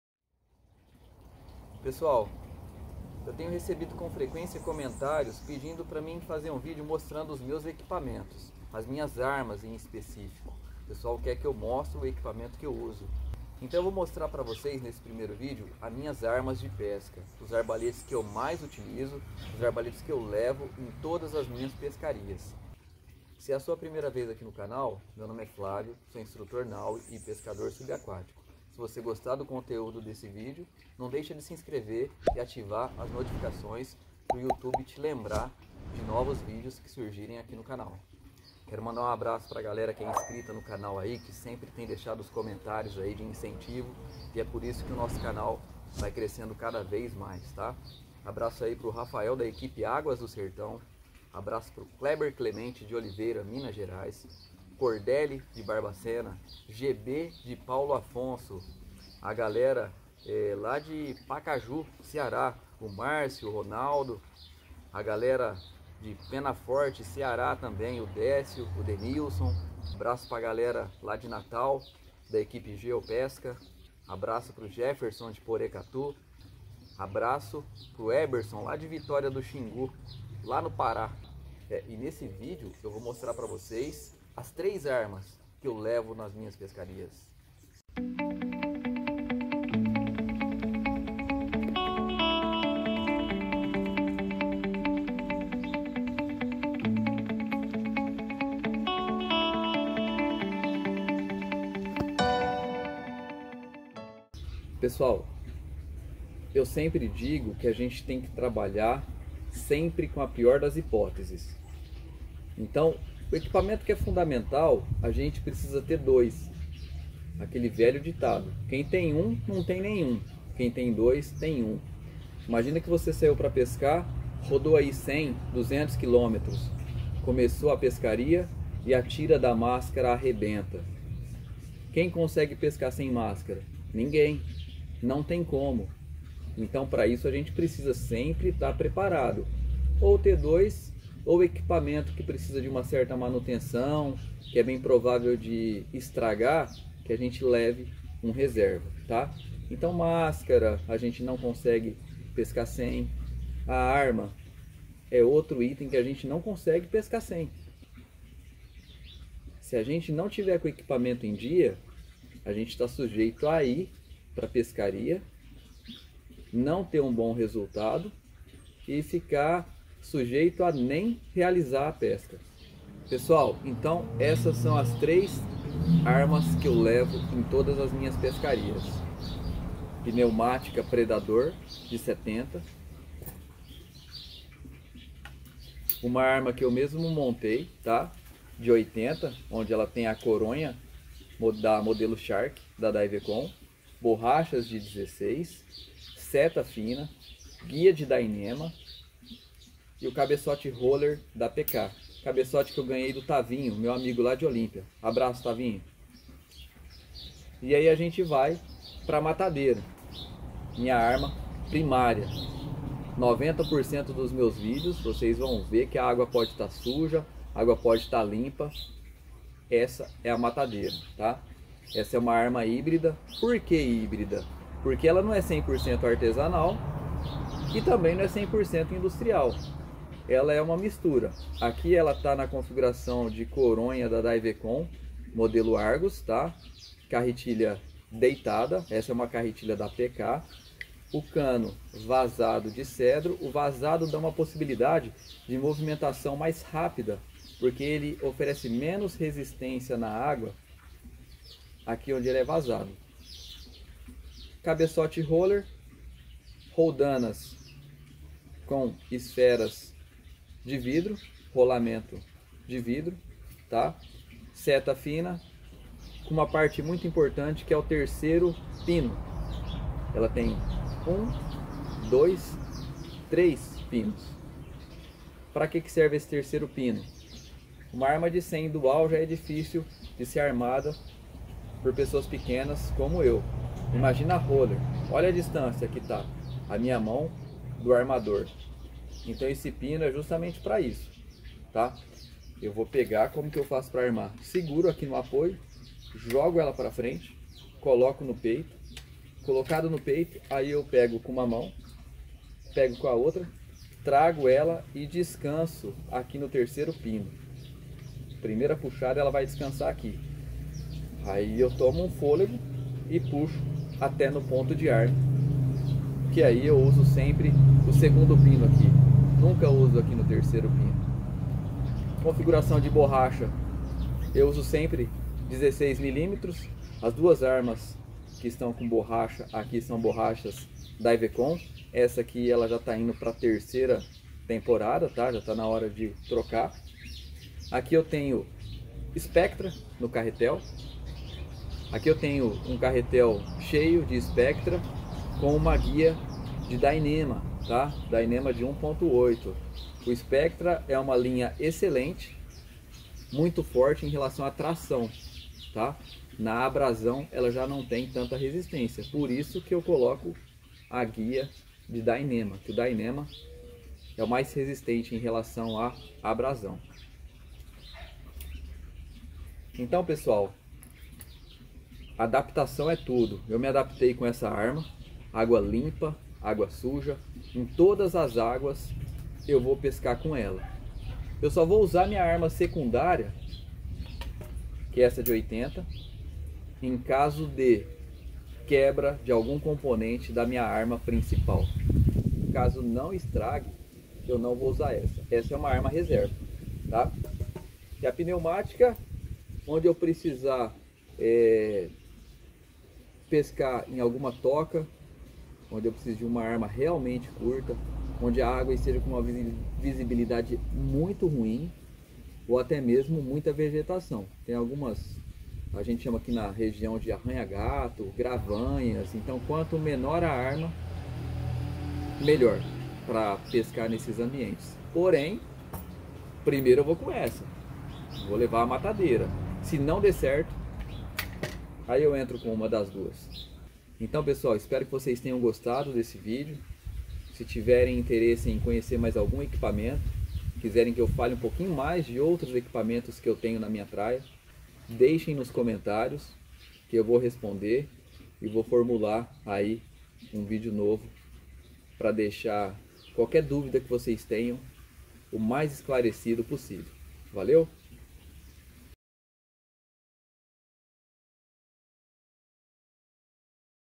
Hunter's Pesca Sub - Armas de Pesca Subaquática - ARBALETE/PNEUMÁTICA
Neste vídeo mostro as três armas de pesca sub que me acompanha em todas as pescarias.